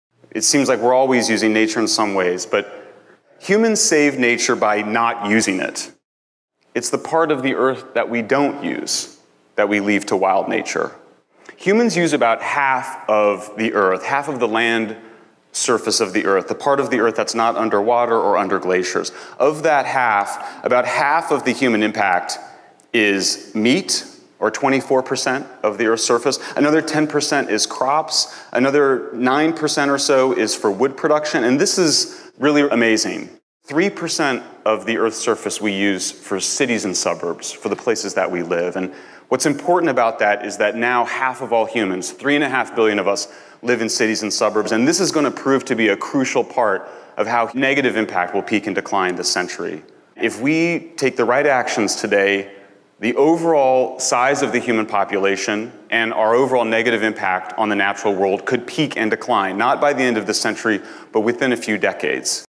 5h33m35s15f Humans Save Nature By Not Using It - Michael Shellenberger - TR2016a
It seems like we're always using nature in some ways, but, human save nature by not using it. It's the part of the Earth that we don't use that we leave to wild nature.

Michael Shellenberger:
Humans use about half of the Earth- half of the land surface of the Earth- The part of the Earth that's not underwater or under glaciers. Of that half, about half of the human impact is meat- or 24% of the Earth's surface. Another 10% is crops. Another 9% or so is for wood production. And this is really amazing, 3% of the Earth's surface we use for cities and suburbs- for the places that we live. And what's important about that, is that now half of all humans three-and-a-half billion of us, live in cities and suburbs- and this is going to prove to be a crucial part of how negative impact will peak and decline in this century.

If we take the right actions today, the overall size of the human population, and our overall negative impact on the natural world could peak and decline- not by the end of the century, but within a few decades.